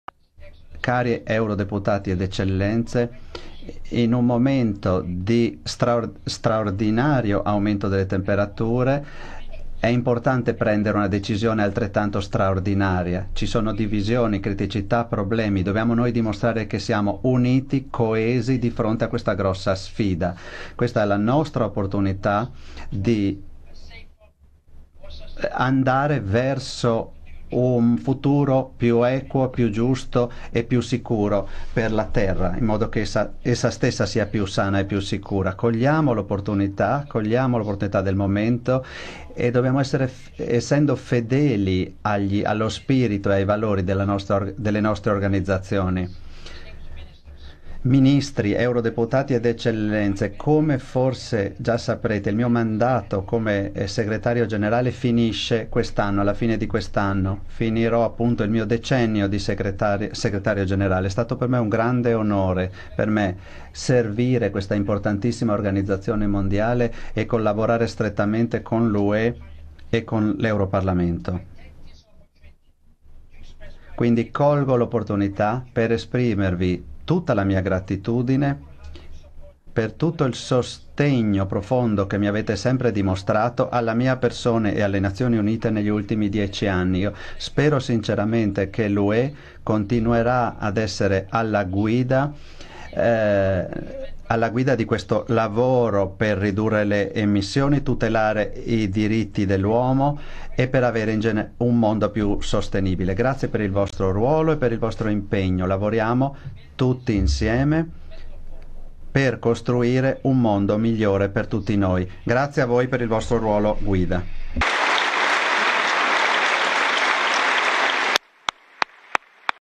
Ban Ki-moon: "Dall'Unione Europea impegno per un mondo più sostenibile"
(Agenzia VISTA) - Bruxelles, 4 Ottobre 2016 - Il Parlamento Ue, riunito in sessione plenaria a Strasburgo, ha dato l'ok alla ratifica da parte dell'Unione Europea dell'accordo di Parigi sul clima. Alla cerimonia di ratifica presenti il segretario generale dell'ONU Ban Ki-moon, il Presidente della Commissione UE Jean-Claude Juncker e il Presidente del Parlamento UE Martin Schulz. "Oggi abbiamo varcato la prima soglia per l'entrata in vigore dell'Accordo di Parigi sul clima - ha detto Ban Ki-moon - Dobbiamo essere uniti e coesi di fronte a questa sfida, abbiamo l'opportunità di un futuro più giusto ed equo per una terra più sana e sicura". "Abbiamo 62 paesi che hanno deciso di far parte dell'Accordo di Parigi - ha aggiunto - Capisco il processo decisionale dell'UE, e lo rispetto, approvo la decisione dei ministri dell'Ambiente dell'UE di agire con sollecitudine. Vi invito ad approvare questo percorso il prima possibile. C'e' la possibilita' di fare la storia, bisogna garantire il futuro dell'Accordo di Parigi consentendo la sua entrata in vigore in tempi rapidi. In nome dell'umanita' vi invito a una ratifica veloce". Il segretario generale dell'ONU ha quindi concluso il suo intervento ringraziando l'UE per il suo "ruolo guida". / EBS